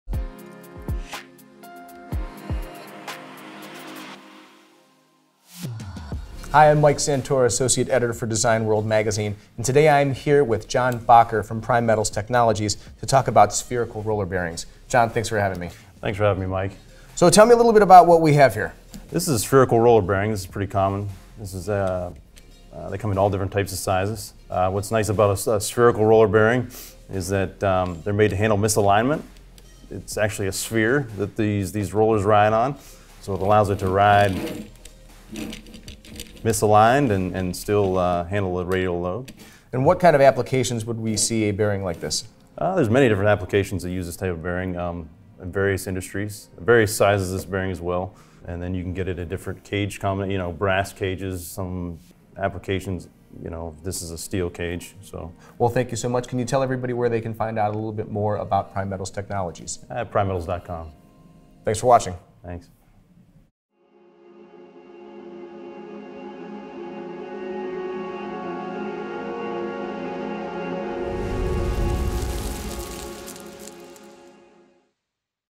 Spherical roller bearing basics: What do I need to know?
In this video, Bearing Tips.com Editor Mike Santora speaks with John Bacher, Bearings Manager for Primetals Technologies, at the company’s facility in Warren, OH. Bacher covers the basics of spherical roller bearings and goes into detail on some of the features unique to this ubiquitous mechanical component.